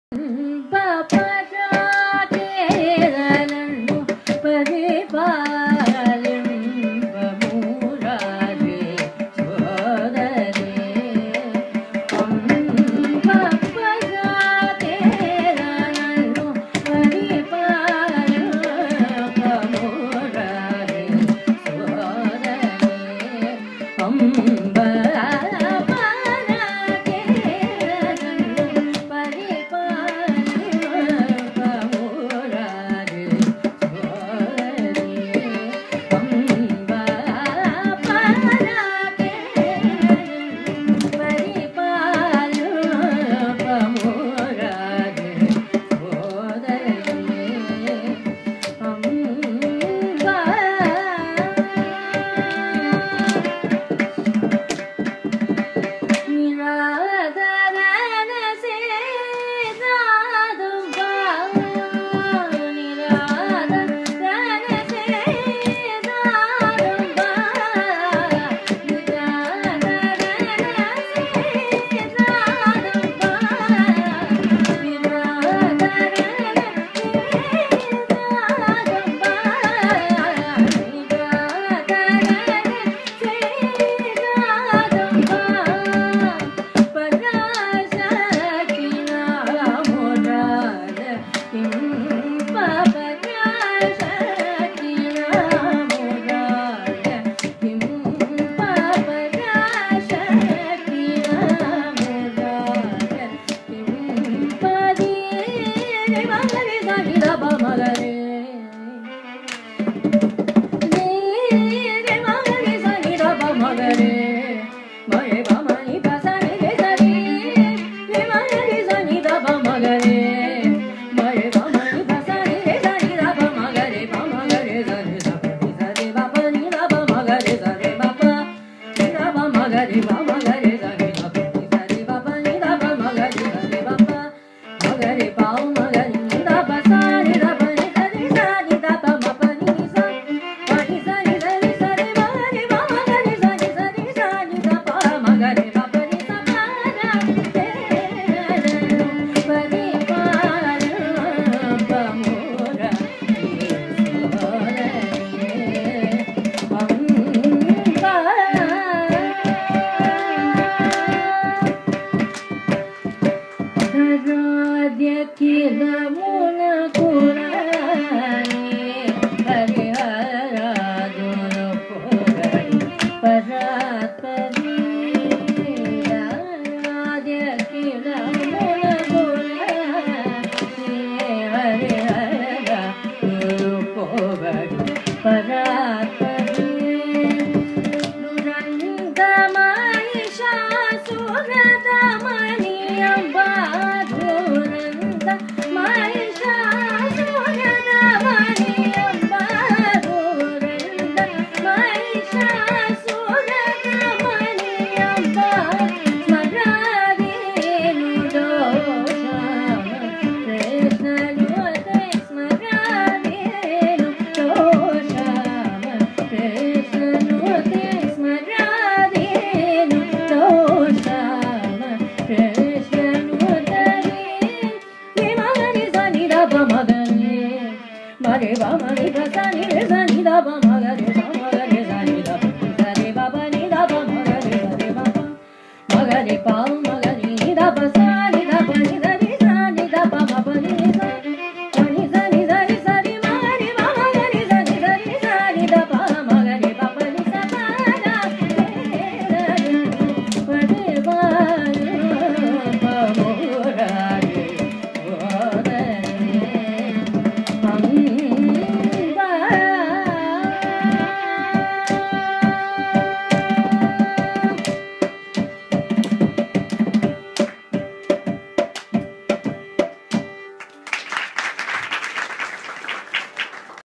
Carnatic concert - Malvika (vocal), Susheela Narasimhan(vio
At Sivamani Residence(Sunnyvale, California) on the occasion of Dusshera Concert Series, 2013.